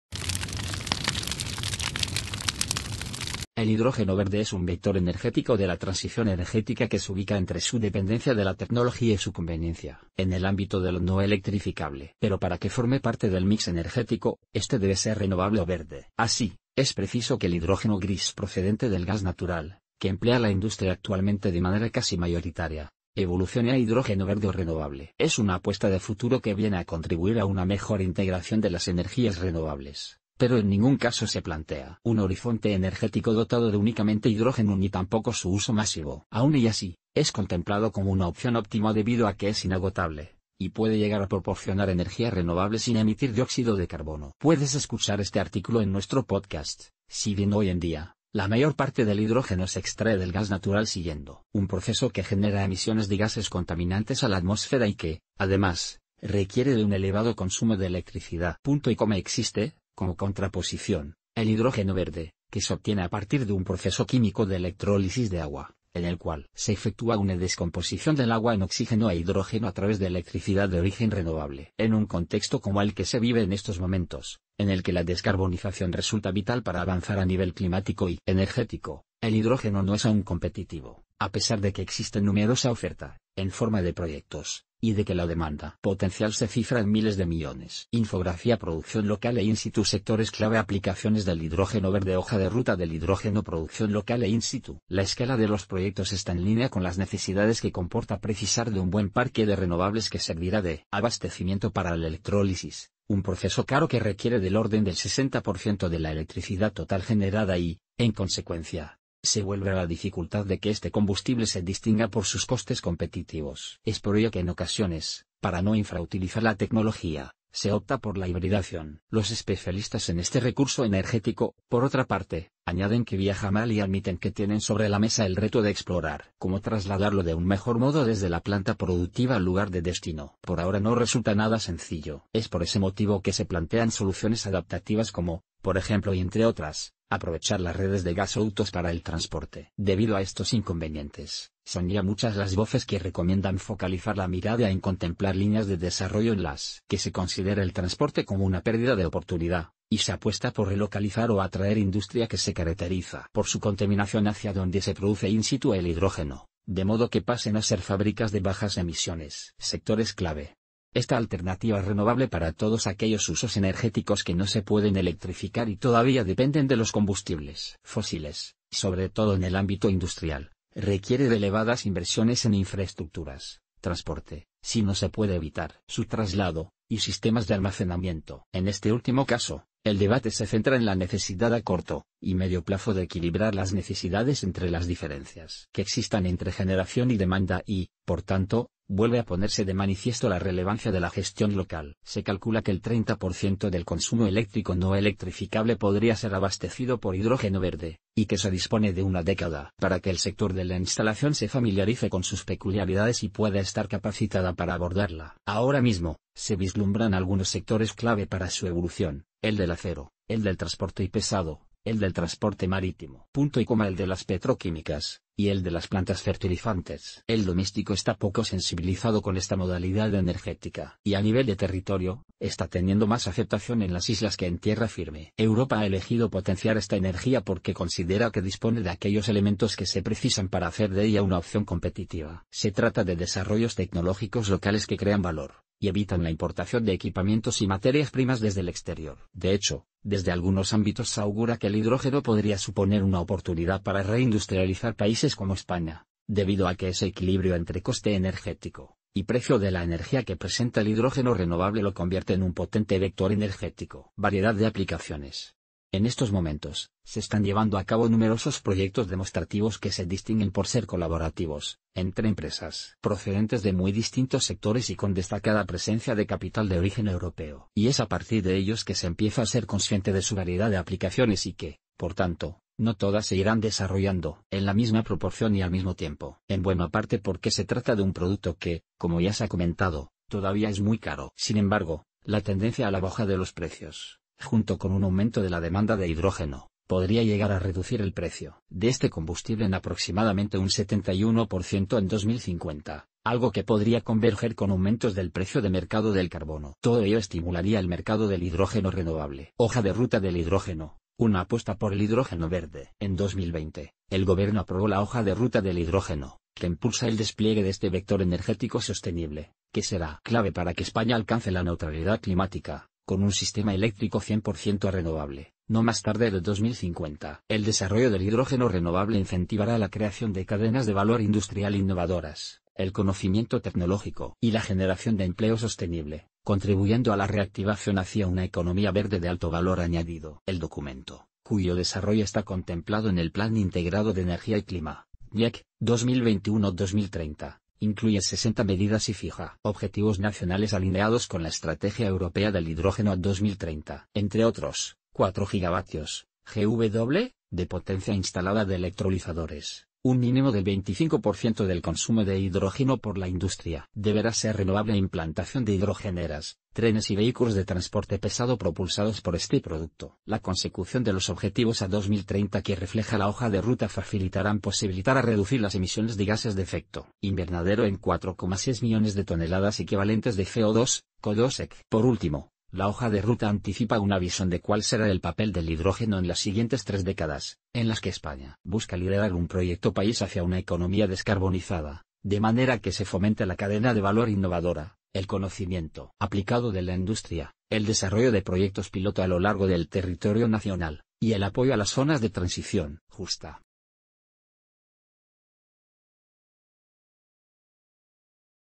El hidrógeno verde como vector energético - Infografía
Hidrógeno verde, vector de la transición energética que se ubica entre su dependencia de la tecnología y su conveniencia en el ámbito de lo no electrificable.